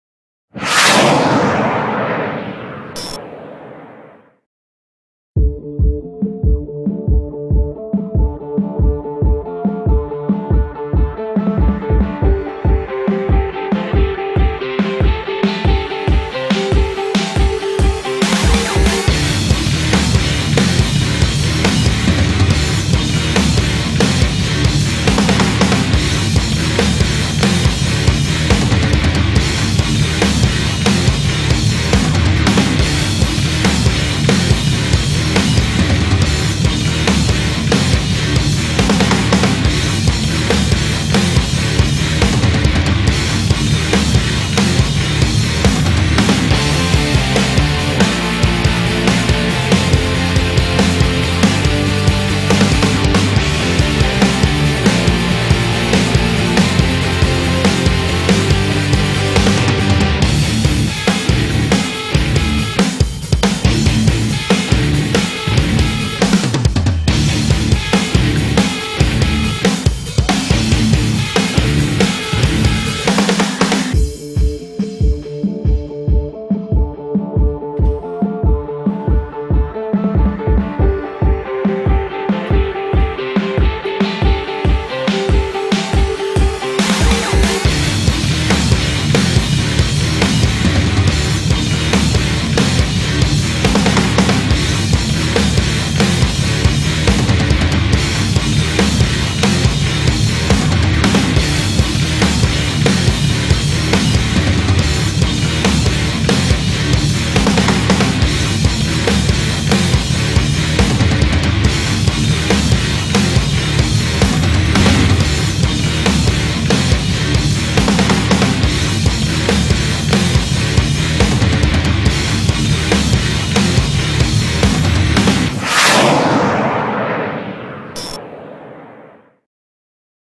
RI 6 Industrial - Automatic Rotor Straightening machine - Schnell Spa
Automatic rotor straightening machine for precision cut-lengths of cold drawn wires. Ideal for all types of wires, mild to high-alloy and for all applications where strict length tolerances (±0,1 mm), high quality straightness and excellent surface finish is required.